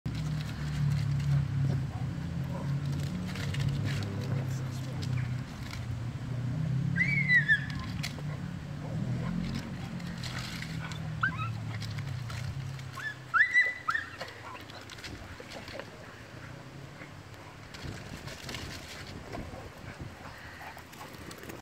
Hilarious Pit Bulls Work Together To Get Ball Out Of Tree!
So close... Yet so far... 🤣💥🐶